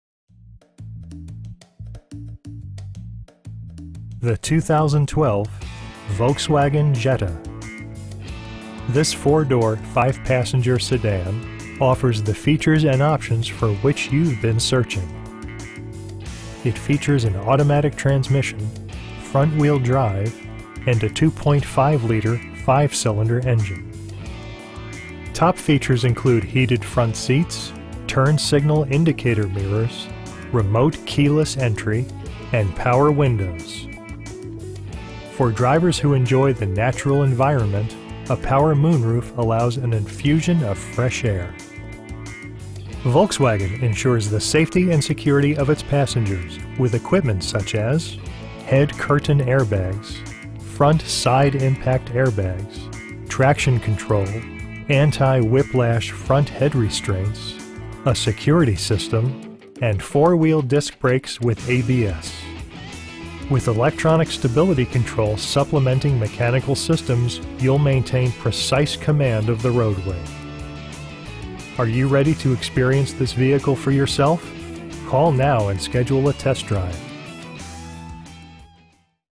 2012 Volkswagen Jetta SE in Indianapolis, IN 46240
Tom Wood Volkswagen
4610-B  E. 96th St.

The 2012 Volkswagen Jetta. This 4 door, 5 passenger sedan offers the features and options for which you've been searching. It features an automatic transmission, front-wheel drive, and a 2.5 liter 5 cylinder engine. Top features include heated front seats, front bucket seats, air conditioning, tilt steering wheel, turn signal indicator mirrors, remote keyless entry, cruise control, and power windows. For drivers who enjoy the natural environment, a power moon roof allows an infusion of fresh air. Volkswagen ensures the safety and security of its passengers with equipment such as: head curtain airbags, front SIDE impact airbags, traction control, anti-whiplash front head restraints, a security system, and 4 wheel disc brakes with ABS. With electronic stability control supplementing mechanical systems, you'll maintain precise command of the roadway. Are you ready to experience this vehicle for yourself?